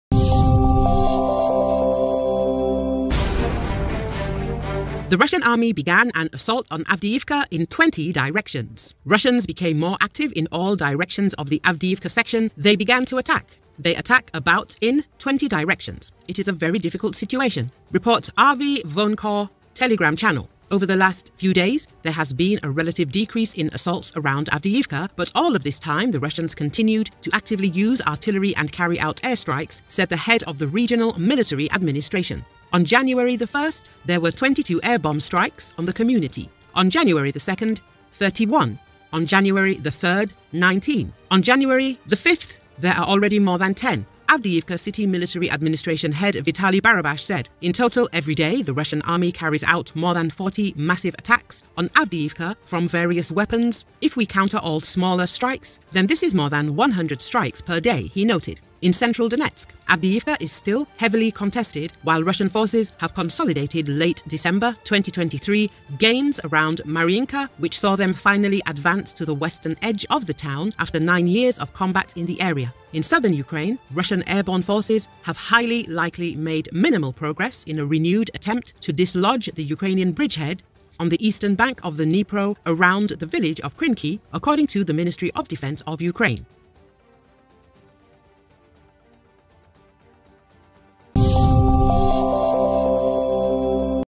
Russian troops launch assault on Avdiivka in 20 directions
“Russians became more active in all directions of the Avdiivka section, they began to attack. They attack about in 20 directions. It is very difficult situation,” reports RVvoenkor telegrame  channel.
Over the last few days, there has been a relative decrease in assaults around Avdiivka, but all this time the Russians continued to actively use artillery and carry out airstrikes, said the Head of the regional military administration.
"On January 1 there were 22 air bomb strikes on the community. On January 2 - 31, on January 3 - 19. On January 4 there were 17, onjanuary there are already more than ten," Avdiivka City Military Administration Head Vitaliy Barabash said.
“In total, every day the Russian army carries out more than 40 massive attacks on Avdiivka from various weapons, if we count all smaller strikes, then this is more than 100 strikes per day”, he noted.
"In central Donetsk, Avdiivka is still heavily contested, while Russian forces have consolidated late December 2023 gains around Marinka, which saw them finally advance to the western edge of the town after nine years of combat in the area.
In southern Ukraine, Russian airborne forces have highly likely made minimal progress in a renewed attempt to dislodge the Ukrainian bridgehead on the eastern bank of the Dnipro around the village of Krynky", according to the Ministry of Defence of Ukraine.